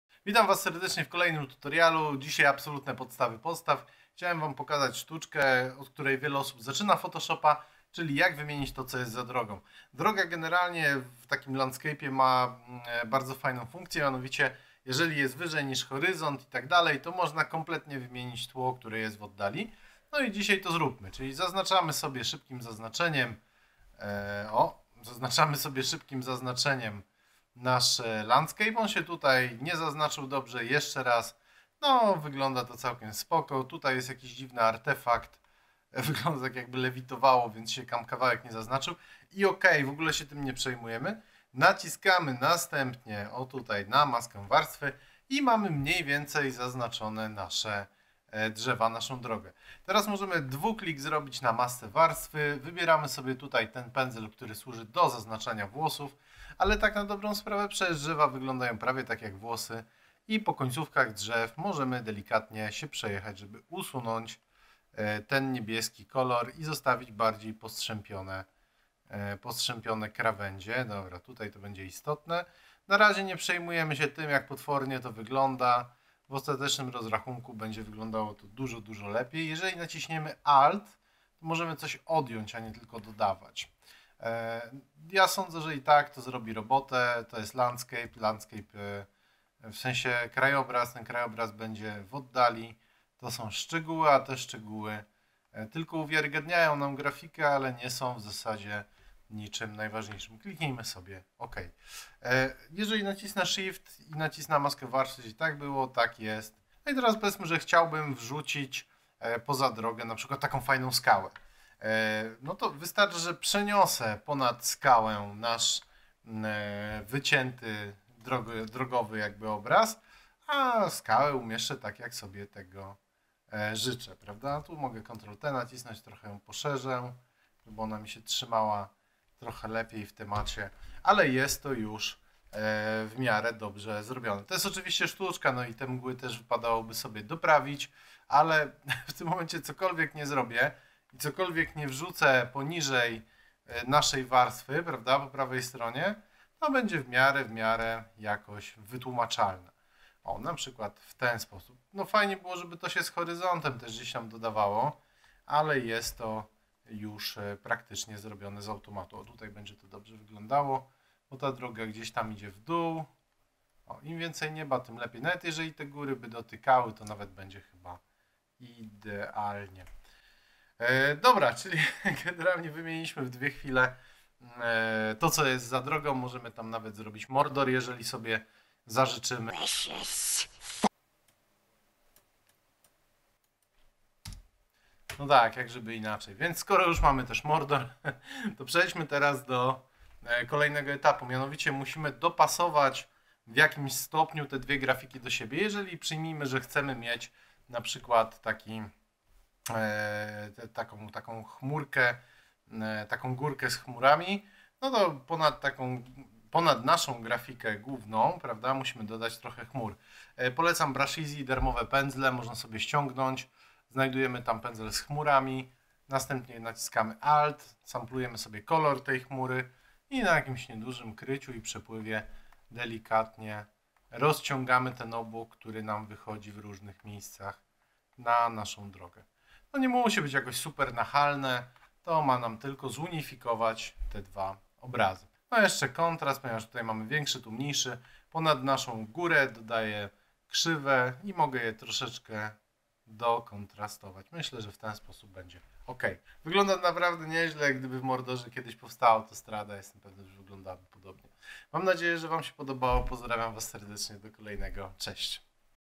Wymiana tła w Photoshopie - ustaw za drogą co zechcesz
Drogi zawsze prowadzą do Rzymu, ale nie w Photoshopie - tutaj możemy podstawić sobie dowolną grafikę i spowodować, że będzie to relatywnie "wiarygodne".